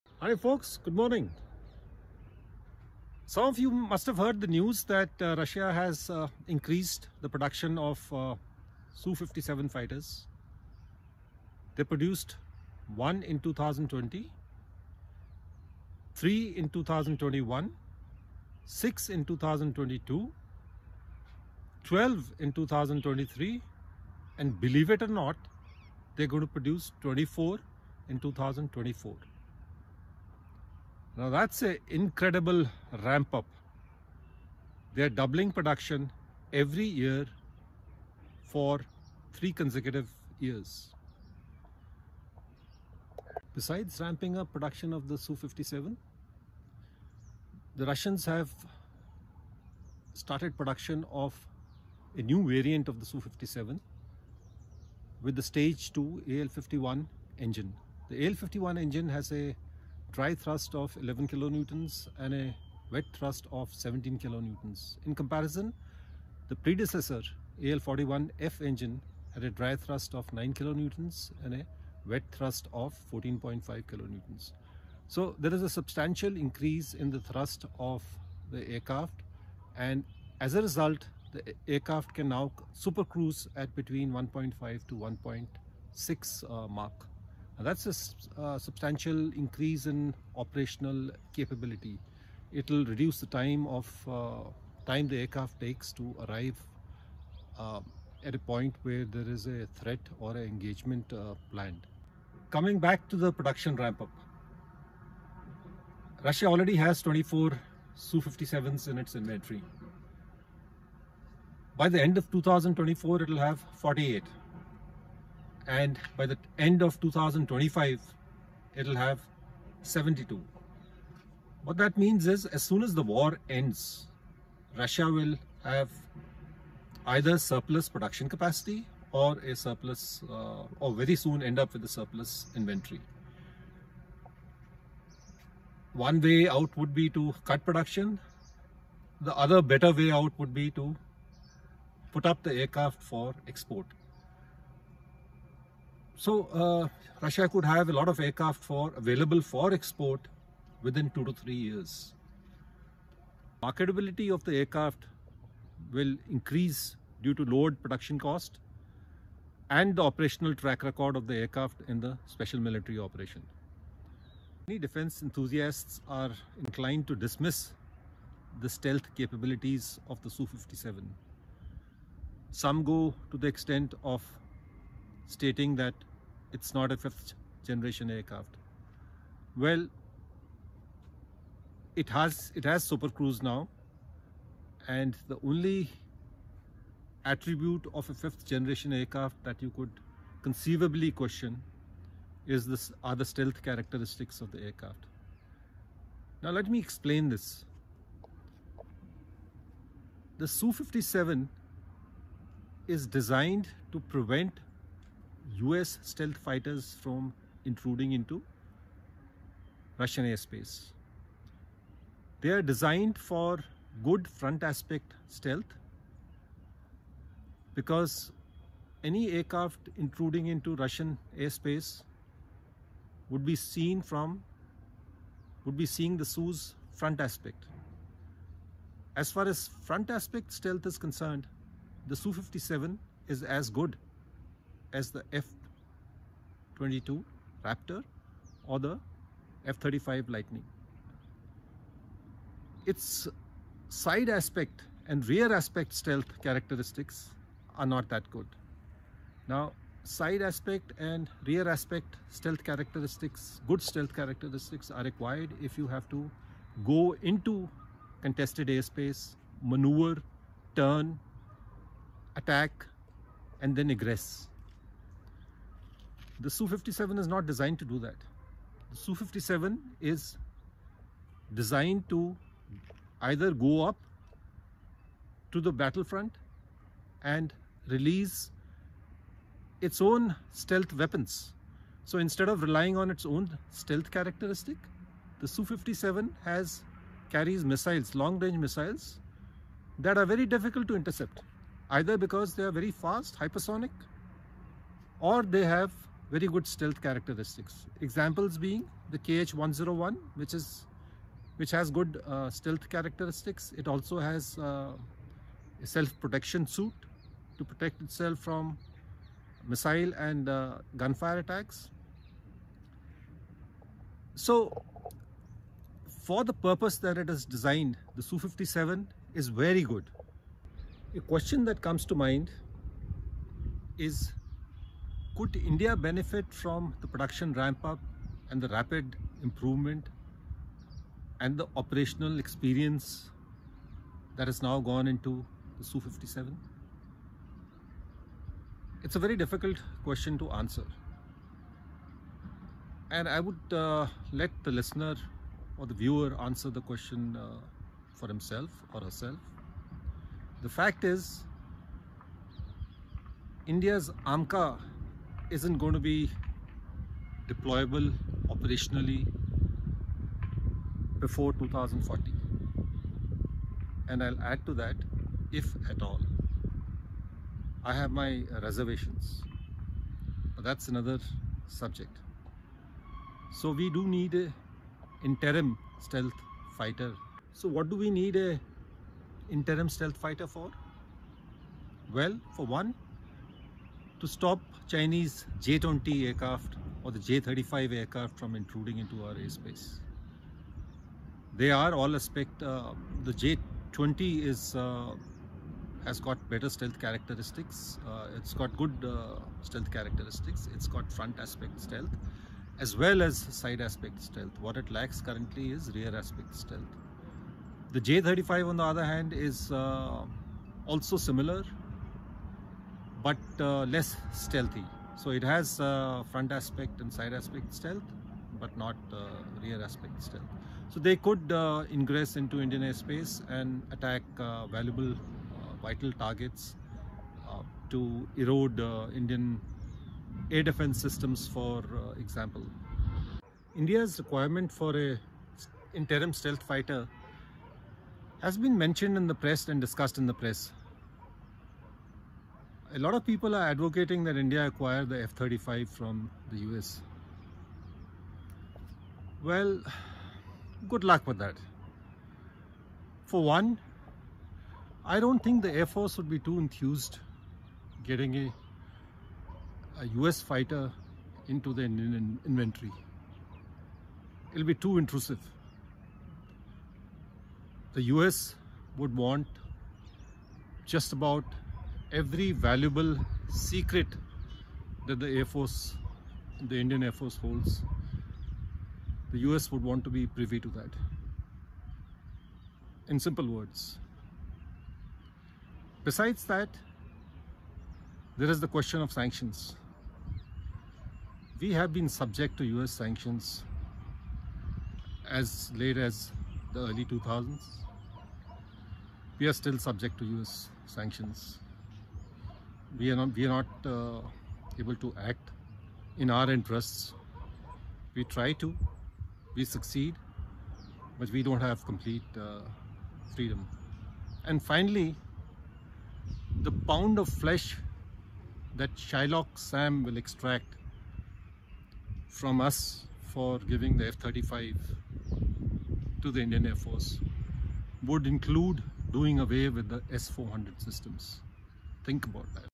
Could India Leverage Russia's Su 57 Production Ramp Up
Russia has doubled its Su-57 production every year for 3 years now. It produced 12 Su-57 fighters in 2013 and reportedly plans to produce 24 in 2024 bringing its 2024 year end inventory to 24.

When Russia's Special Military Operation (SMO) ends in a year or two, Russia will likely have a production capacity far in excess of its needs. It would be looking for exports offering a competitive price due to increased production; also features honed during the course of the SMO. Could India leverage the position that Russia ends up in to its own advantage?

The video footage in the video is courtesy UAC.